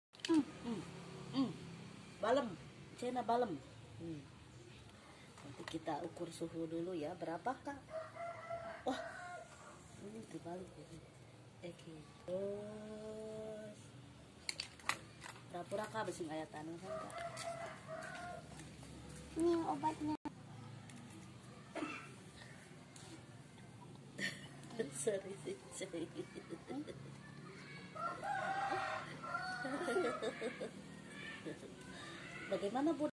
main dokter dokteran😄
bermain dokter dokteran mishel sama kaka